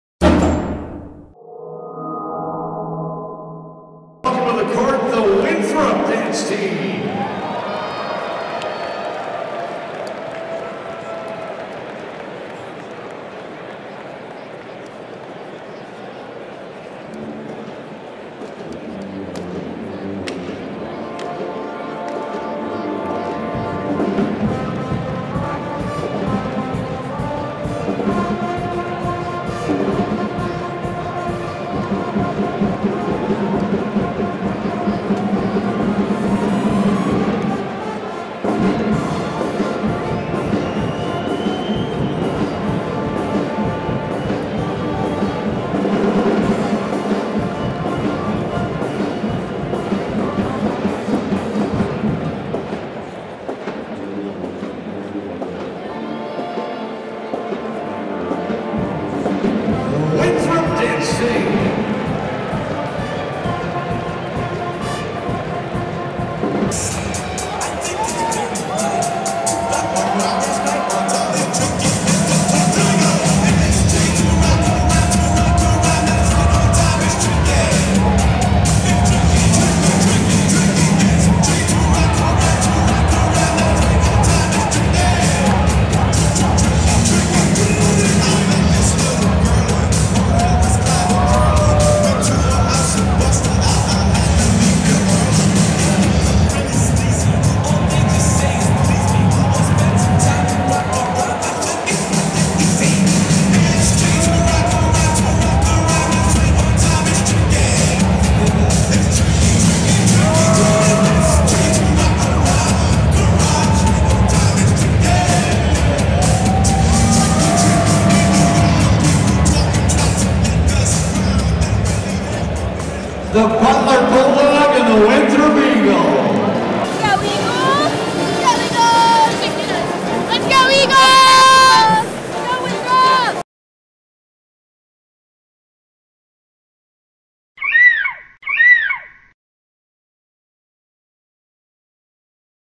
Spirit Squad at NCAA Tourney
The Men's Basketball Team wasn't the only Winthrop squad on the court today check out the Garnet Girls & Lil Stuff